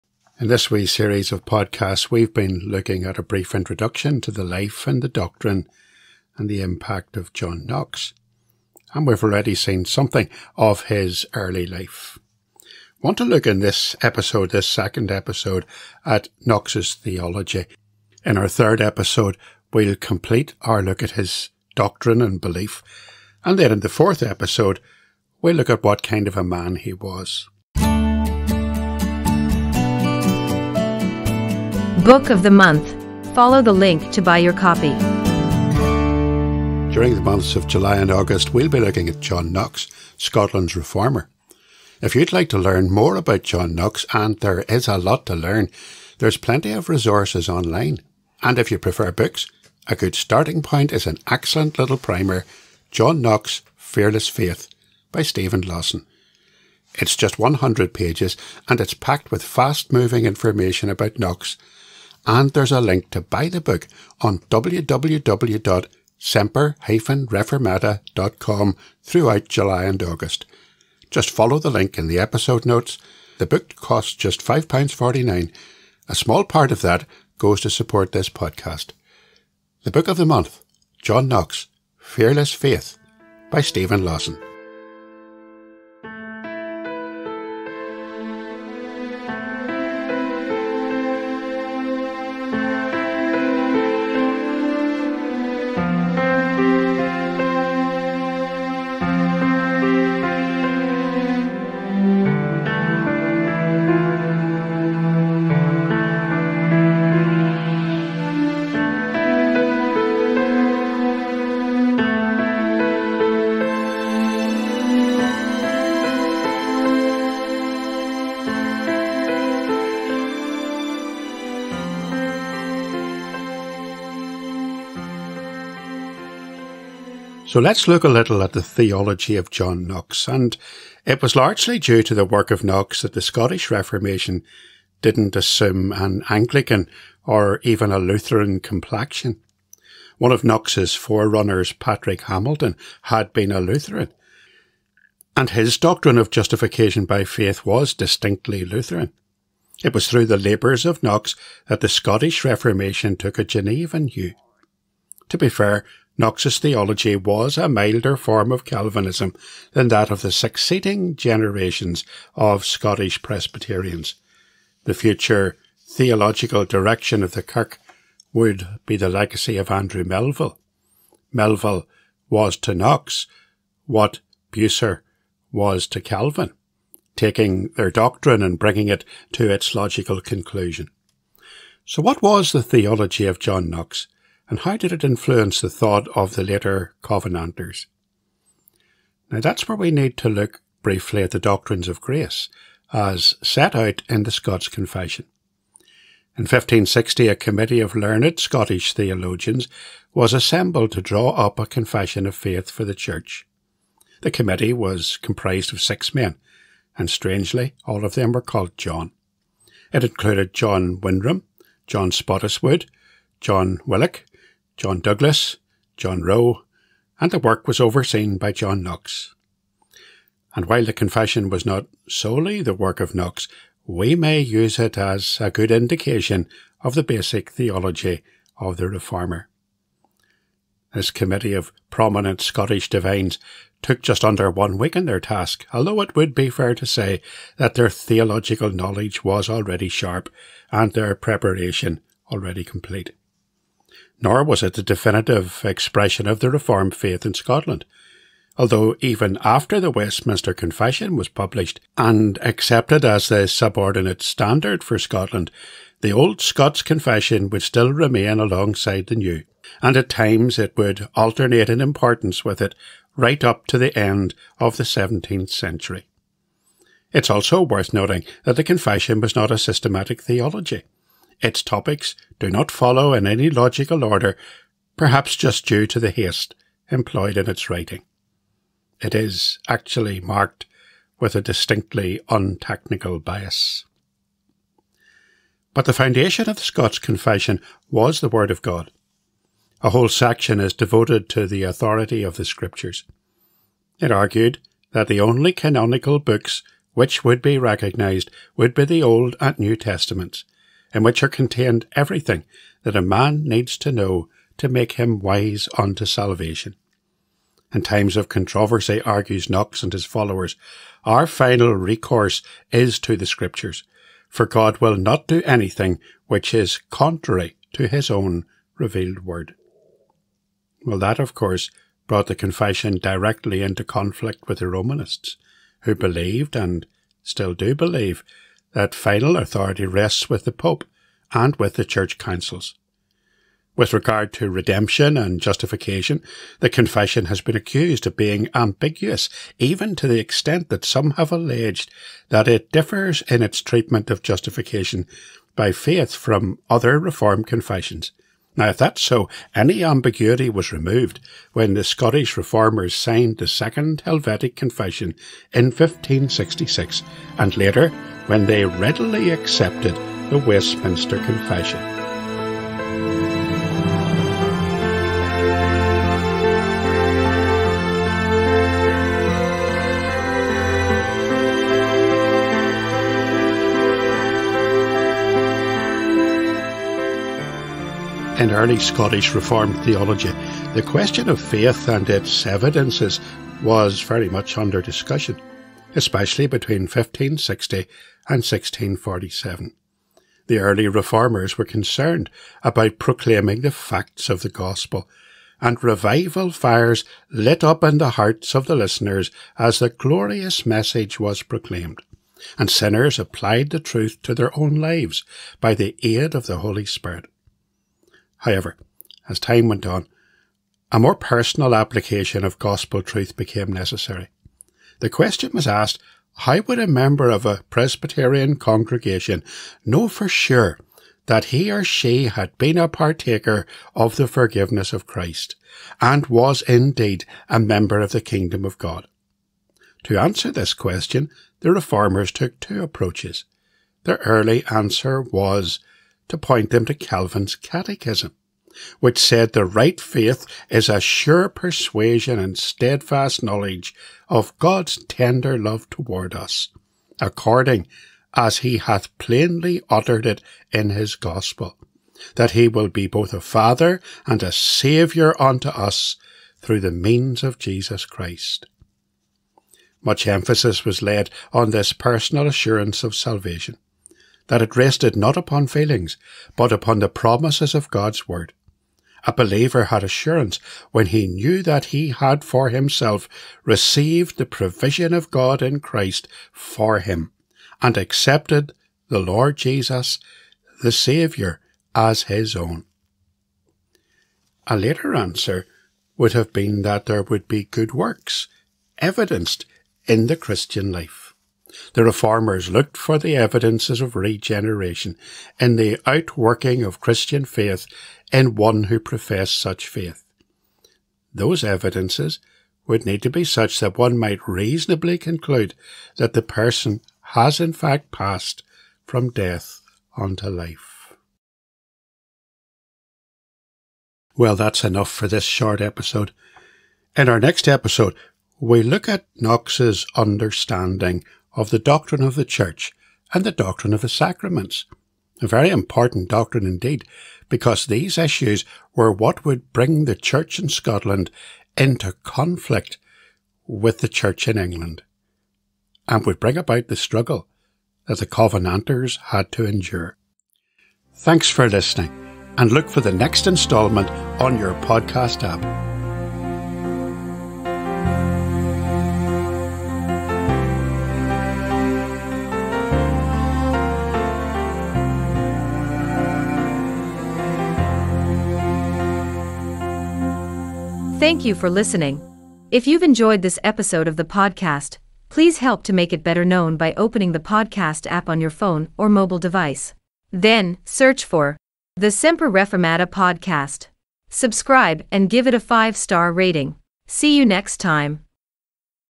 John Knox and the Scots Confession - Part 2
Part two of this Summer history series looking at the life and teaching of John Knox, Scotland's reformer. We begin to look at Knox's doctrine and teaching.

"Scotland"

Learn more about Knox.Follow this link to BOOK RECOMMENDATIONS AND PURCHASE OPTIONSBuying books using the links provided will help support the podcast